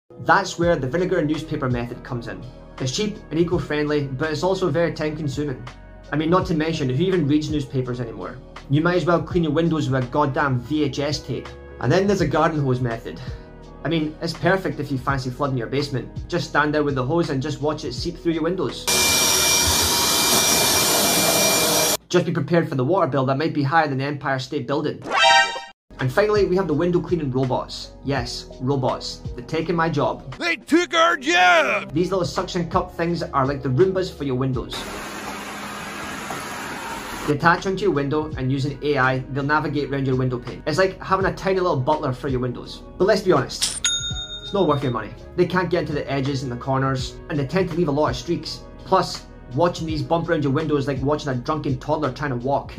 Can AI Clean Your Windows?
Are you tired of the constant struggle of keeping your windows clean? Well, today we're going to be discussing the topic of Robot Window Cleaners, and whether they're really worth the investment.

While these devices may seem like a convenient solution to your window cleaning woes, there are a few things to consider before you make the purchase. For starters, Robot Window Cleaners can be quite expensive, with some models costing upwards of $500. And while they may save you time in the short term, they can also be quite finicky to set up and operate, which may not be worth the hassle for some.

Additionally, Robot Window Cleaners are not always as effective as traditional cleaning methods. They may struggle to clean heavily soiled or hard-to-reach areas, which can lead to streaks and missed spots. And if you have windows with intricate designs or delicate frames, Robot Window Cleaners may not be suitable at all.

Overall, while Robot Window Cleaners may be a tempting solution for those looking to simplify their window cleaning routine, it's important to weigh the costs and benefits before making the investment. So before you rush out to buy one, be sure to do your research and consider whether it's really the right choice for you.